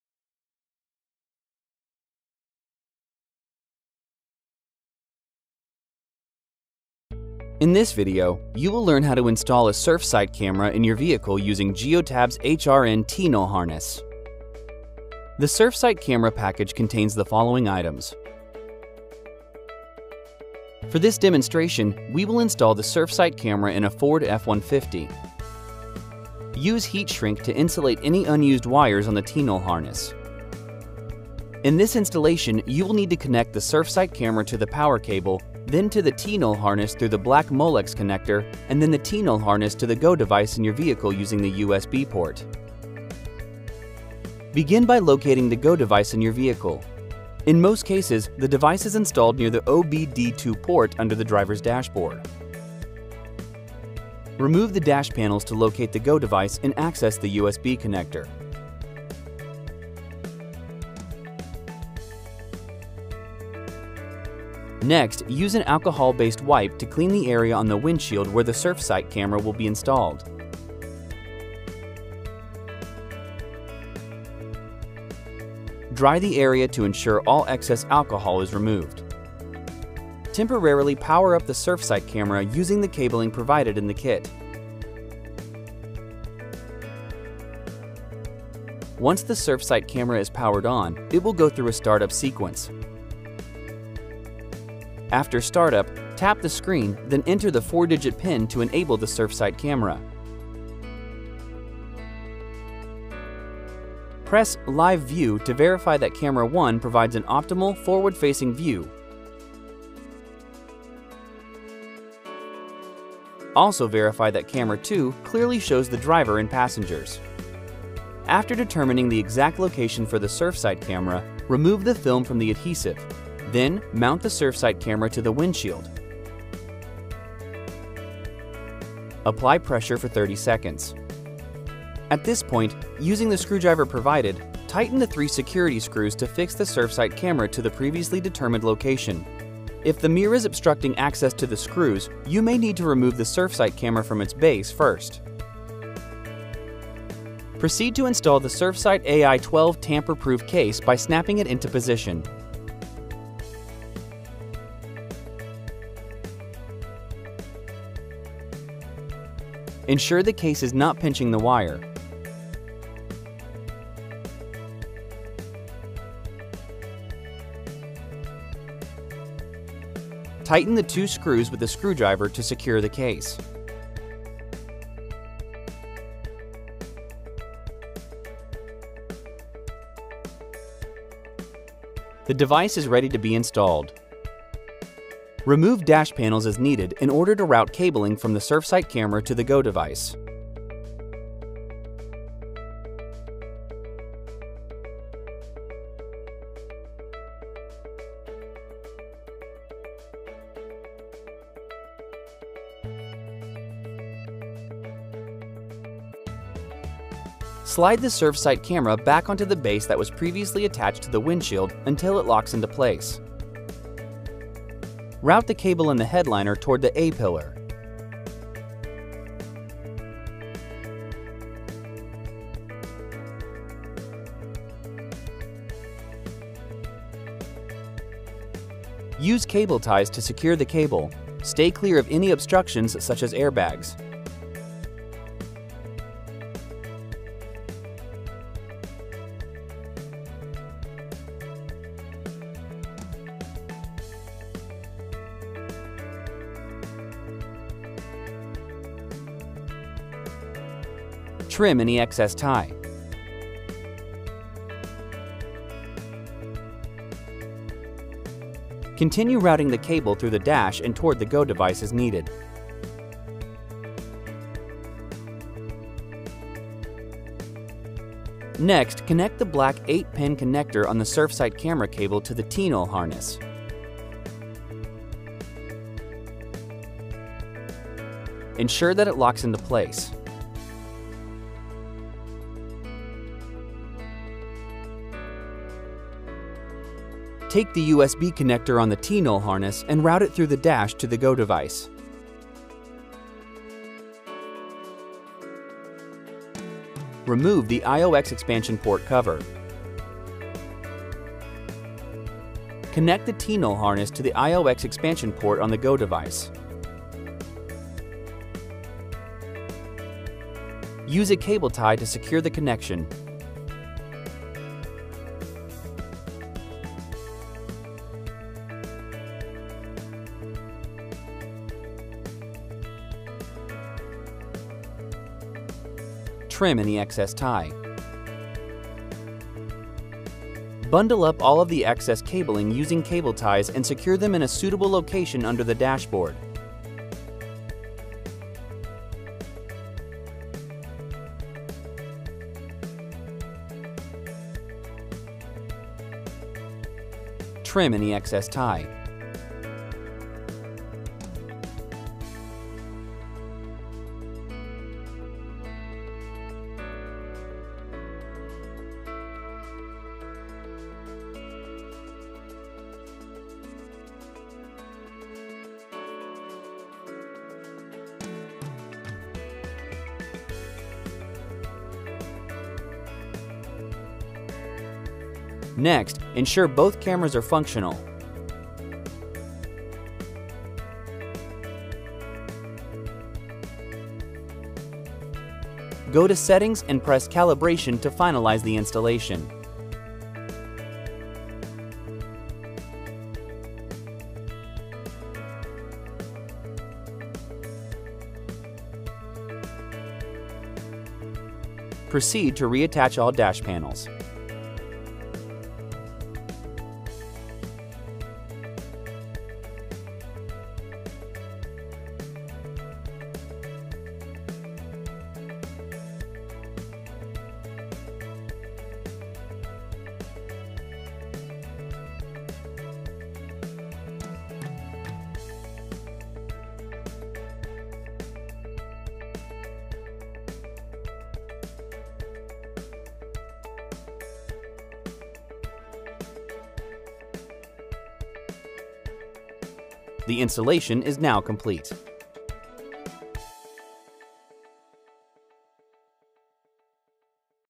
How to Install a Surfsight AI-12 Camera Using Geotab’s HRN-TNULL Harness
In this video, you will learn how to install a Surfsight AI-12 camera in your vehicle using Geotab’s HRN-TNULL harness.

! IMPORTANT: USA Only: Place the enclosed CARB EO identification label sticker in the engine compartment of the vehicle near the Vehicle Emission Control Information label on a smooth and clean surface. EO identification label is required to aid inspection of the vehicle under the California Smog Check program and other applicable smog test programs.